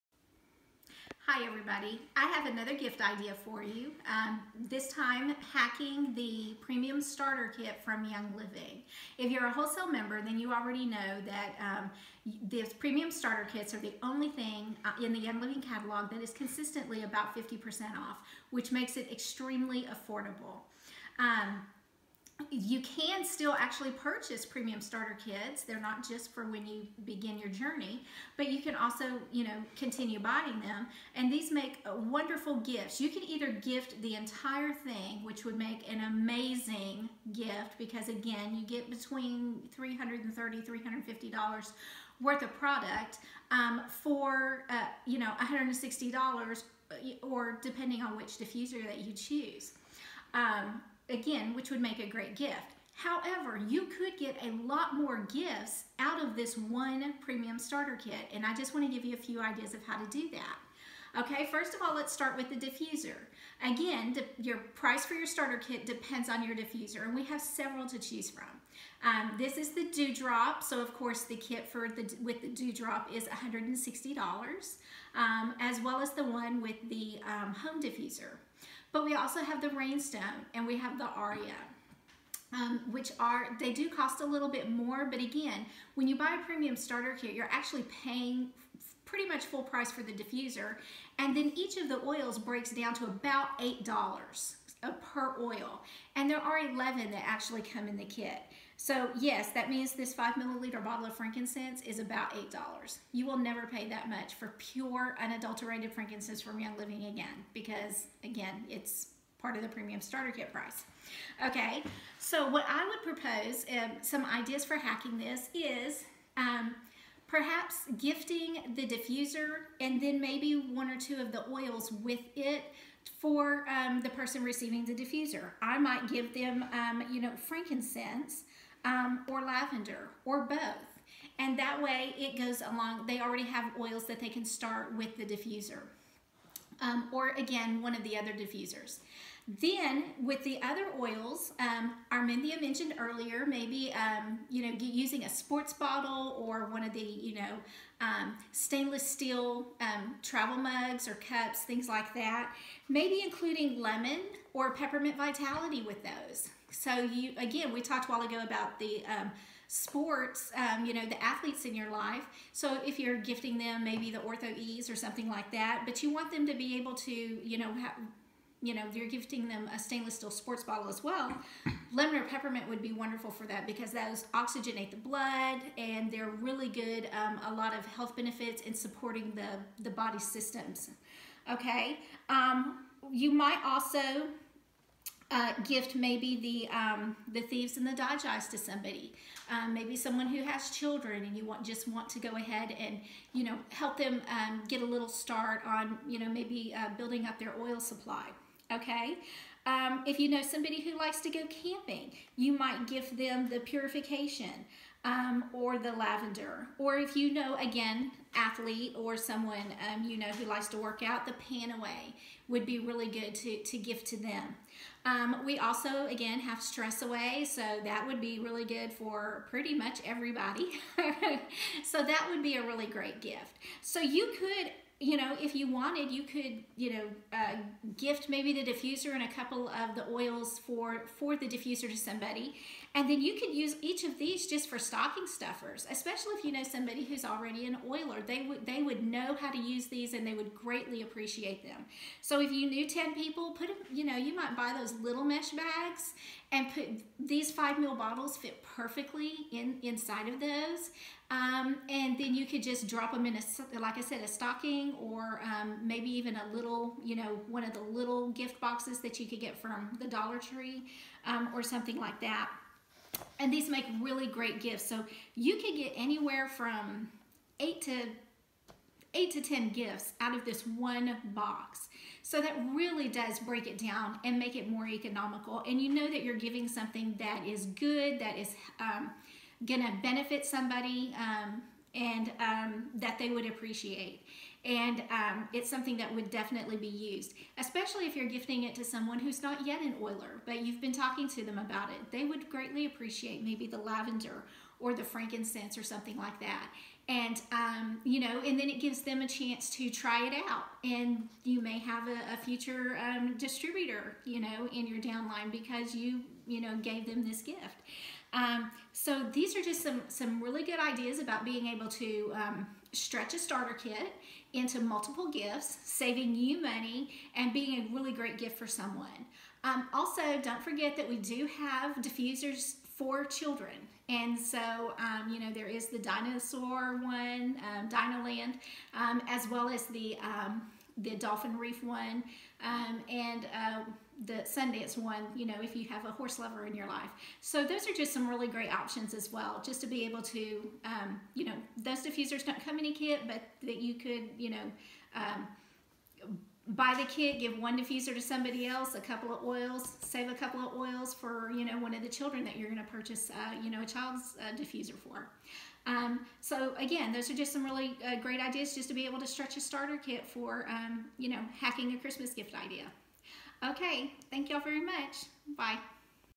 Premium Starter Kit
An extraordinary value that offers a comprehensive introduction to the power of essential oils, the Premium Starter Kit is the perfect option for those who are serious about transforming their lives.

Your Premium Starter Kit Includes:

Dewdrop™ Diffuser
Premium Essential Oils Collection;
Lavender 5-ml
Peppermint Vitality™ 5-ml
Lemon Vitality™ 5-ml
Copaiba Vitality™ 5-ml
Frankincense 5-ml
Thieves® Vitality™ 5-ml
Purification® 5-ml
R.C.™ 5-ml
DiGize™ Vitality™ 5-ml
PanAway® 5-ml
Stress Away™ 5-ml
AromaGlide™ Roller Fitment
10 Sample Packets
10 Love It? Share It! Sample Business Cards
10 Love It? Share It! Sample Oil Bottles
2 NingXia Red® 2-oz. samples
Product Guide and Product Price List
Essential Oil Magazine
Essential Edge
Member Resources
One or more of the following essential oils may be substituted in the event of sourcing constraints or supply considerations: AromaEase™, Citrus Fresh™ Vitality™, Lemongrass Vitality™, Orange Vitality™ and Tea Tree.